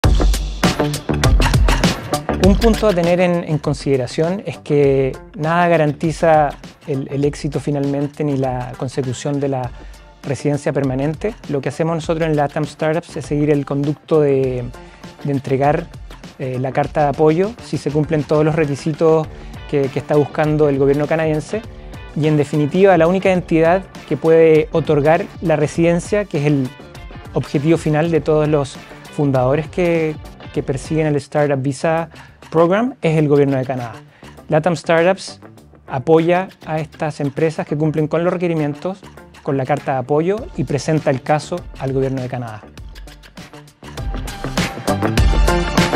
¿Qué es la carta de apoyo en el StartupVisa program?
No es una carta común, es un documento que se maneja a nivel federal con la cual se otorga el apoyo a las empresas que llenan los requisitos para este programa. La carta es uno de cuatro requisitos que se necesitan.